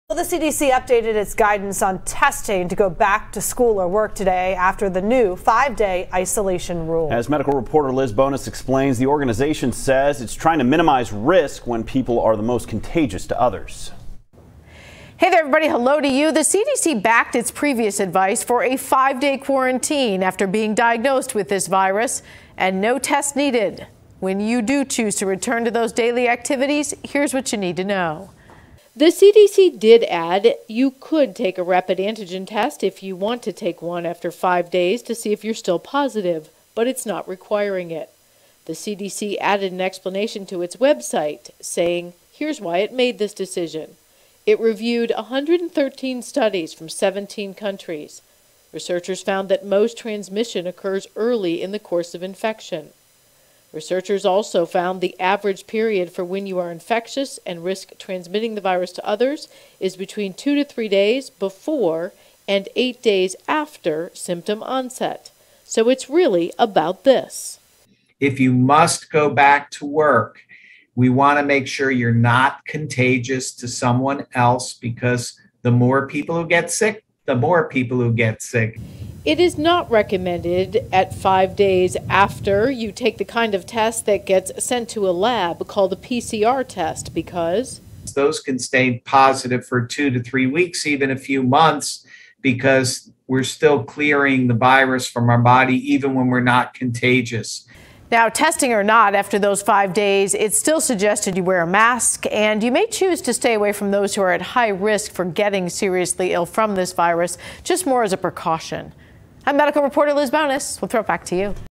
CDC updates guidance on COVID-19 testing after isolation
The CDC Wednesday updated its guidance on testing to go back to work or school after the new five-day isolation rule